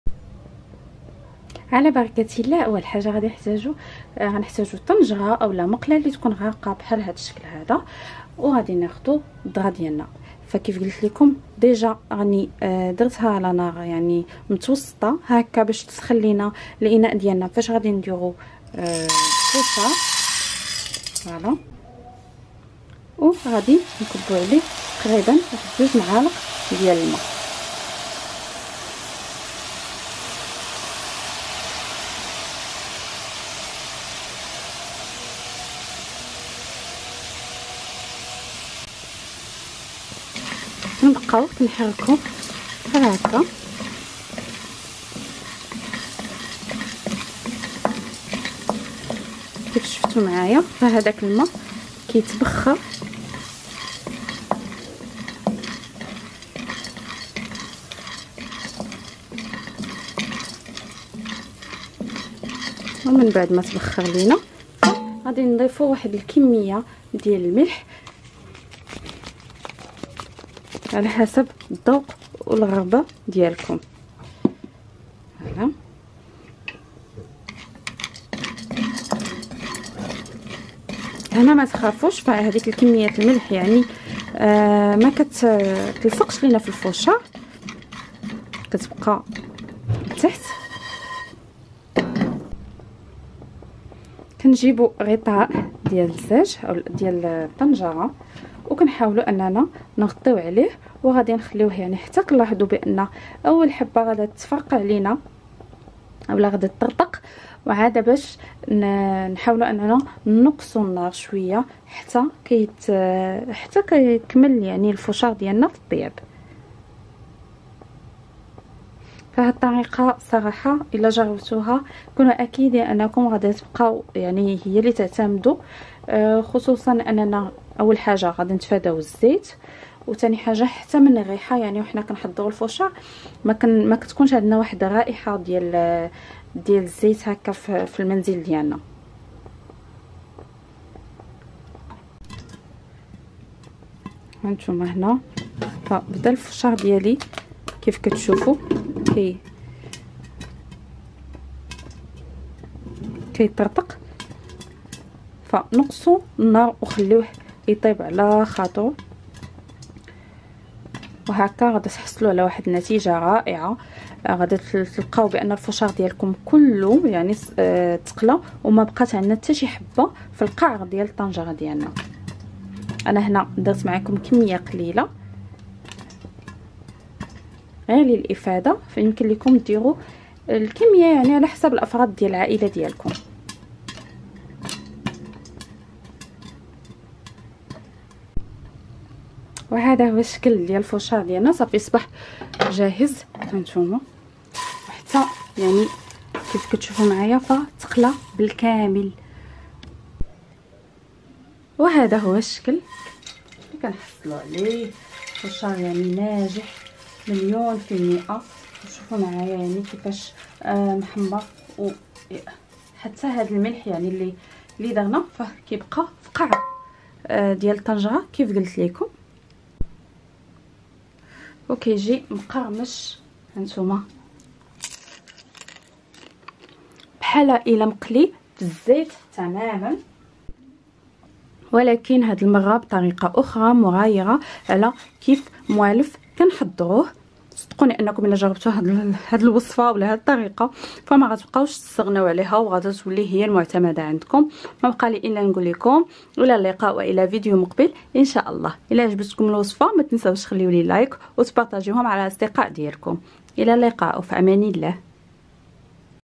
حضري الفشار المقلي في الماء بدون ولا نقطة زيت جربي و حكمي
شكرا على متابعتكم ل قناتي 😍😍

لتواصل معي على انستقرام💕💕